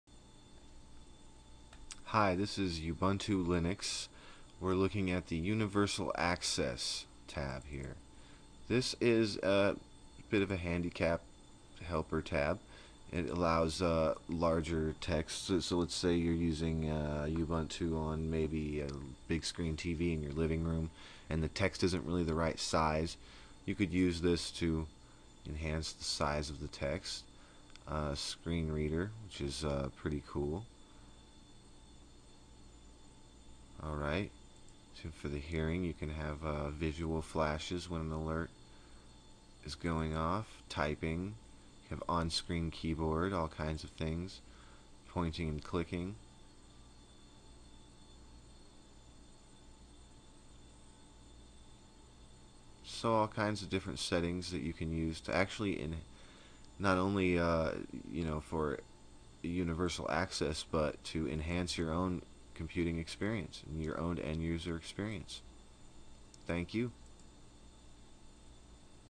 P2.0 Ubuntu Beginners 10 Universal Access
Ubuntu universal access has settings that adjust the computer for visual and hearing impaired.